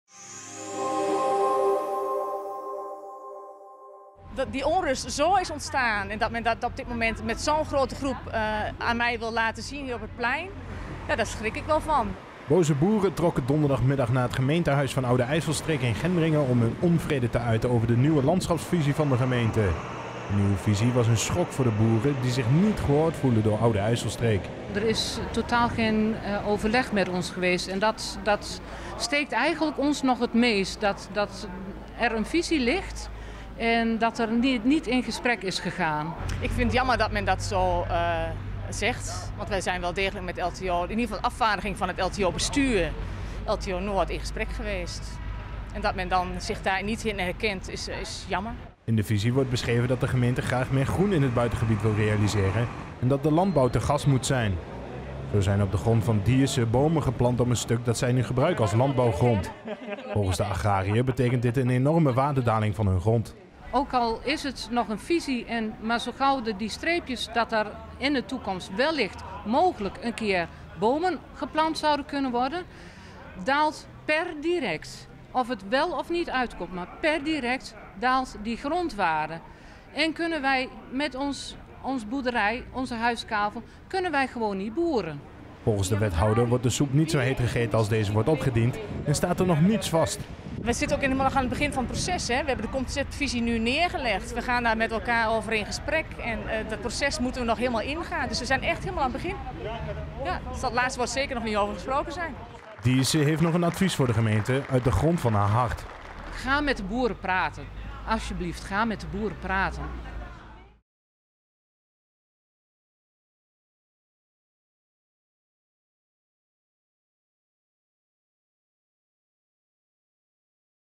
Boeren protesteren tegen vernieuwde landschapsvisie van Oude IJsselstreek
Boze boeren trokken donderdagmiddag naar het gemeentehuis van Oude IJsselstreek in Gendringen om hun onvrede te uiten over de nieuwe landschapsvisie van de gemeente. De nieuwe visie was een schok voor de boeren, die zich niet gehoord voelen door Oude IJsselstreek.